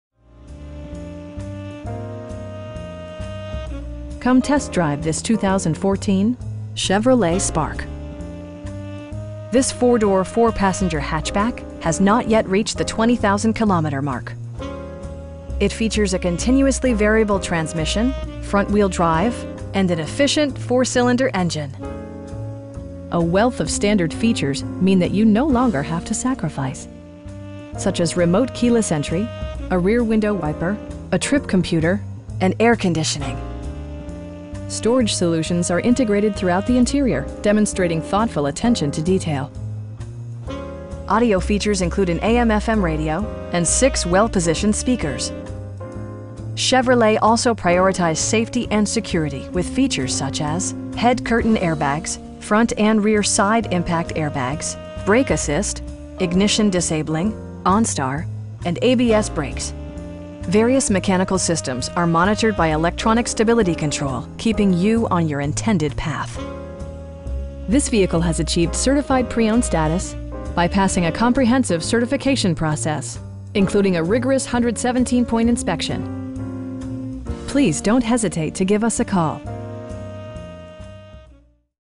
2014 Chevrolet Spark 1LT CVT FWD Auto w/ MyLink
2595 Barnett Highway.

Come test drive this 2014 Chevrolet Spark. This 4 door, 4 passenger hatchback has not yet reached the 20,000 kilometer mark! It features a continuously variable transmission, front-wheel drive, and an efficient 4 cylinder engine. A wealth of standard features means that you no longer have to sacrifice. Such as remote keyless entry, delay-off headlights, a rear window wiper, a trip computer, tilt steering wheel, power door mirrors, cruise control, and air conditioning. Storage solutions are integrated throughout the interior, demonstrating thoughtful attention to detail. Audio features include an AM/FM radio, and 6 well positioned speakers. Chevrolet also prioritized safety and security with features such as: head curtain airbags, front and rear SIDE impact airbags, traction control, brake assist, ignition disabling, onStar, and ABS brakes. Various mechanical systems are monitored by electronic stability control, keeping you on your intended path. This vehicle has achieved Certified Pre-Owned status, by passing a comprehensive certification process, including a rigorous 117 point inspection! We have the vehicle you've been searching for at a price you can afford. Please don't hesitate to give us a call.